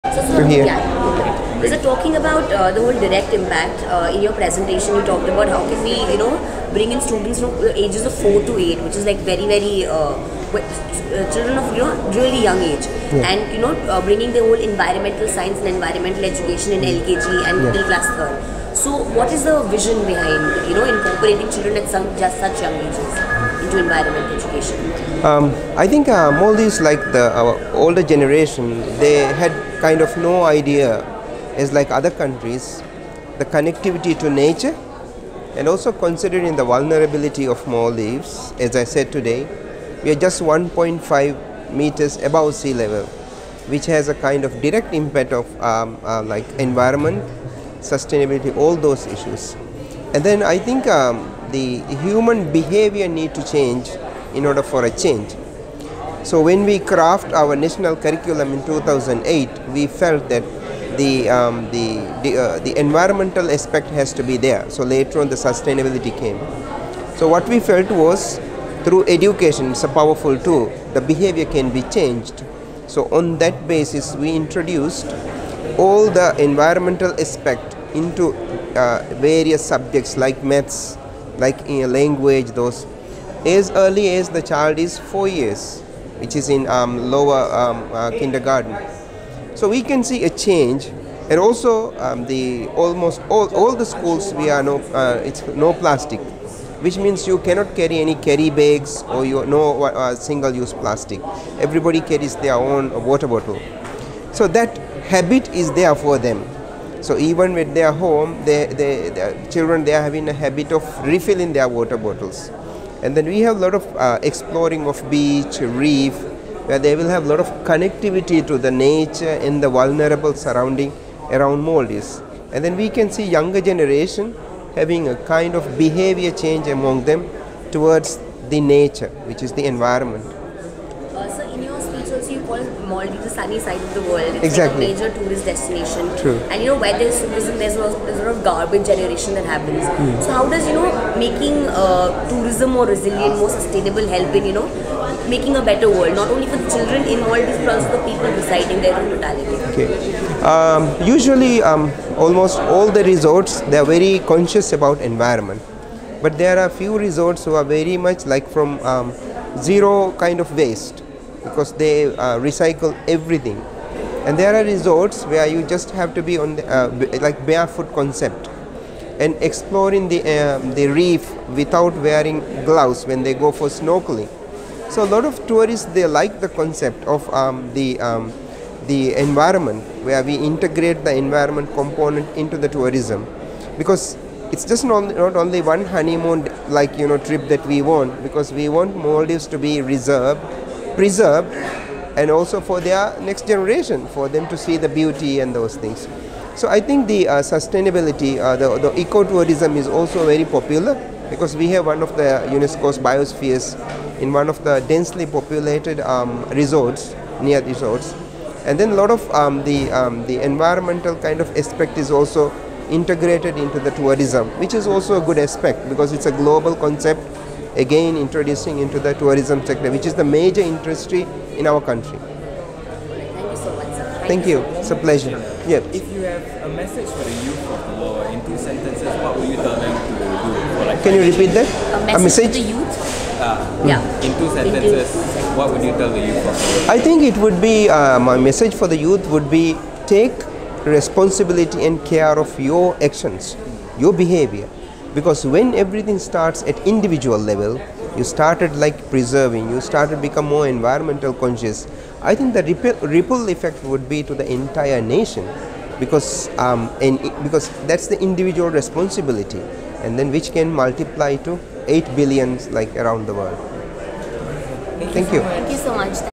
Interview with Ahmed Athif, Ministry of Education, Maldives
Learn about the environmental education of young school children in the Maldives and the effects of tourism on the islands.